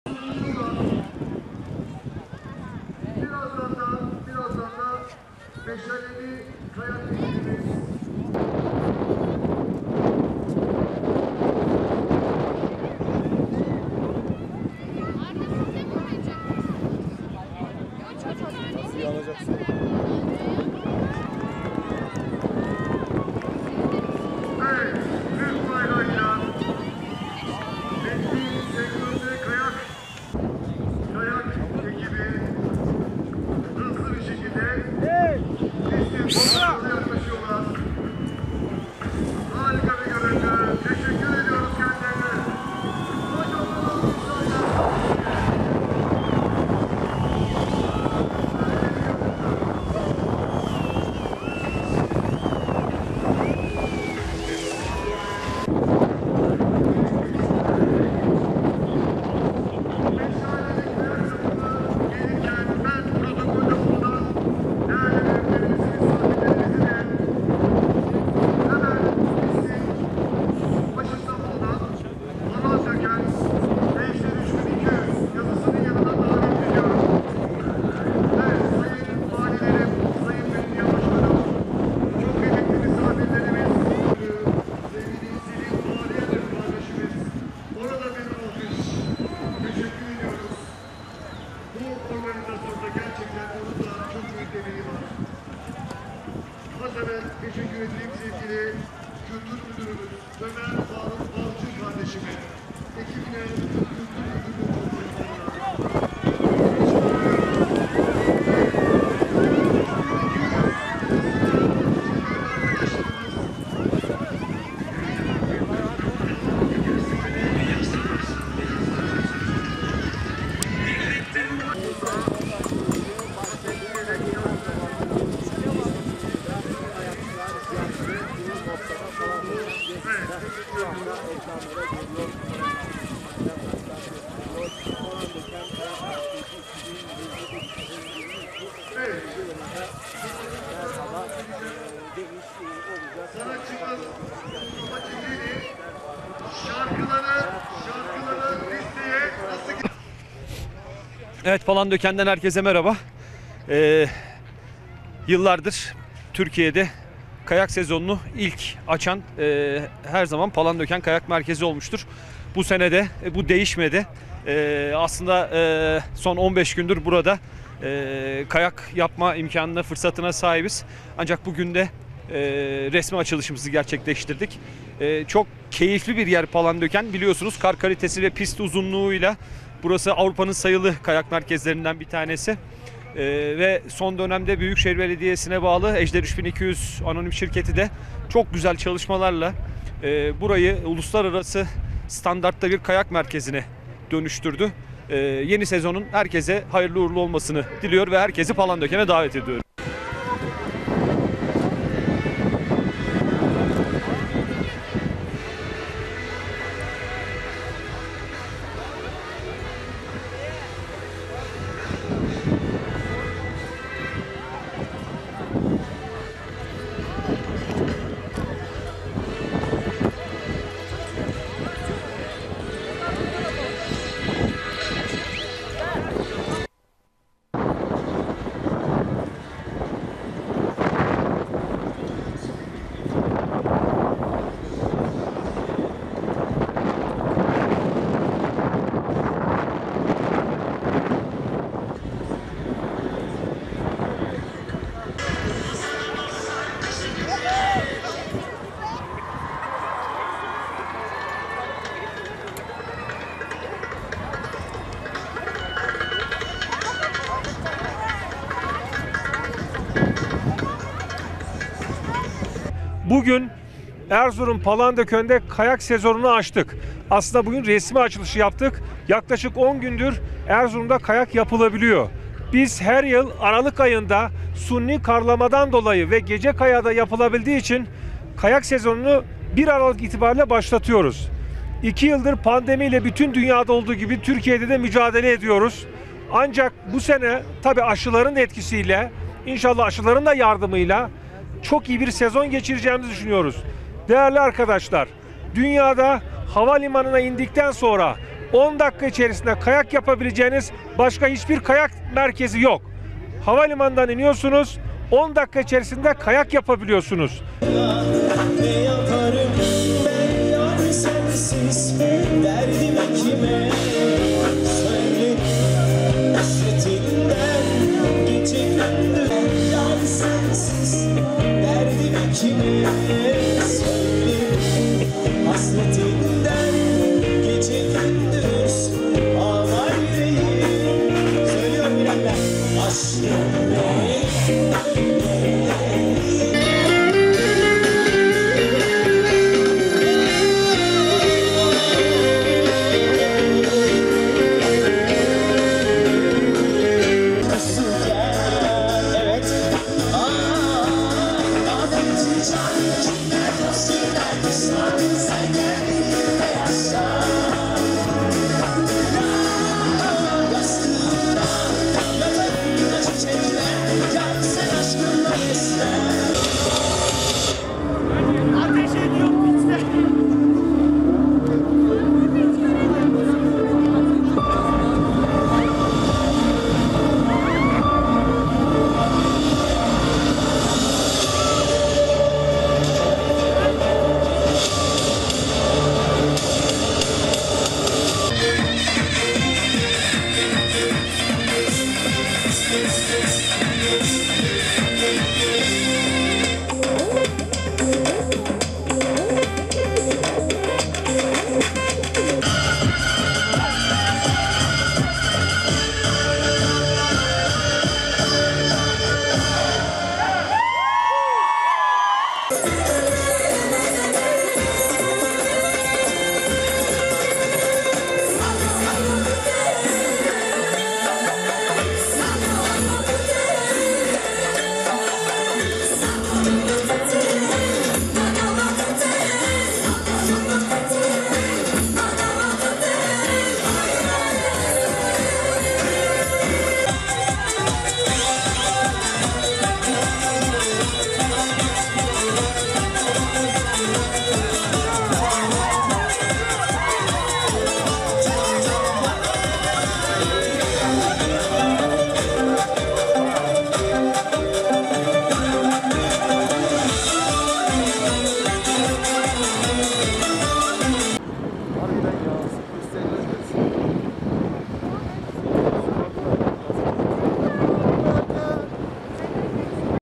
Kayak sezonu Palandöken'de başladı
New York Times Gazetesi tarafından belirlenen dünyadaki 41 kayak merkezi arasında 18. sırada gösterilen Erzurum'daki Palandöken Kayak Merkezi'nde kış sezonu açılışı, meşaleler ve konserler eşliğinde yapıldı.